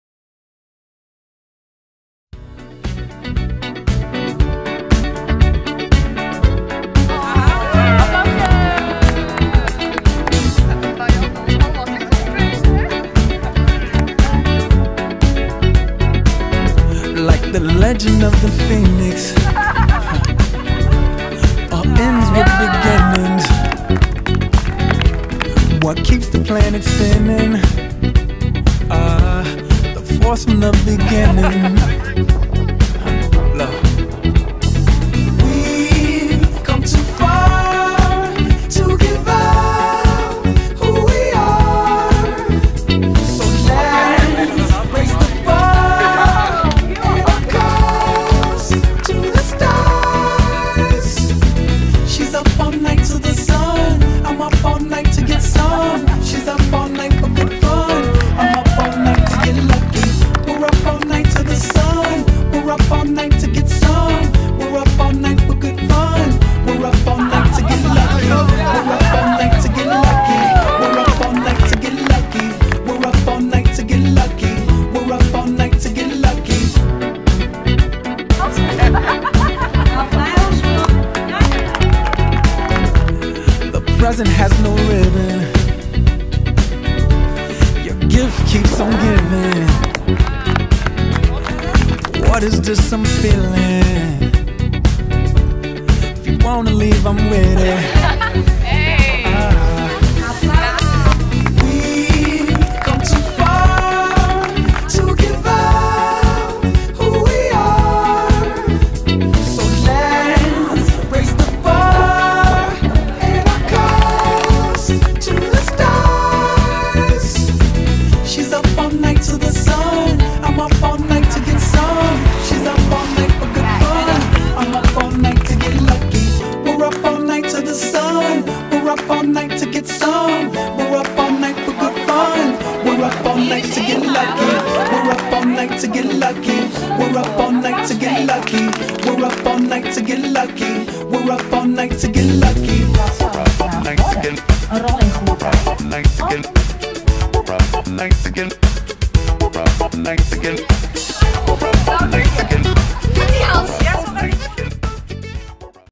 ontvangst groep 8
Bijgaand de aankomst van de vertrekkende leerlingen van C.B.S. De Pream De Knipe op weg naar de premiere van hun eigen film!